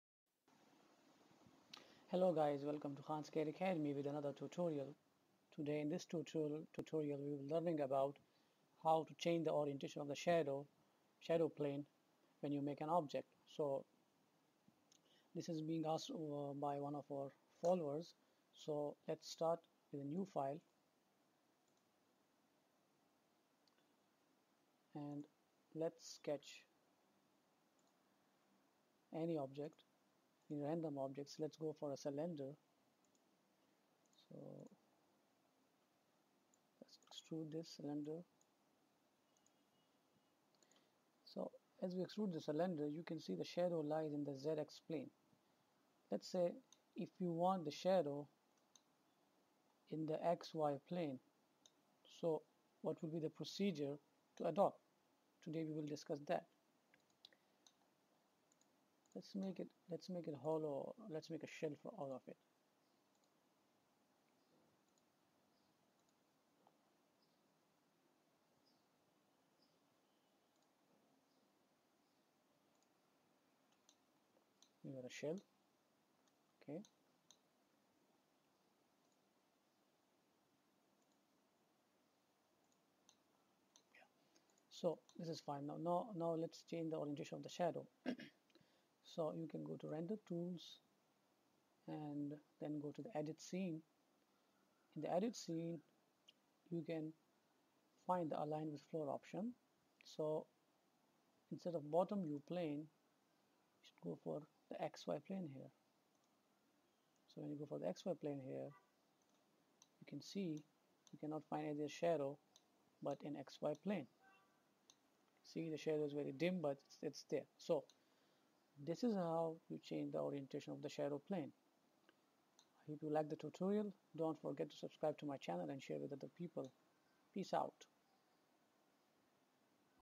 Changing shadow plane in solidworks
In this tutorial you will learn about changing shadow plane for an object.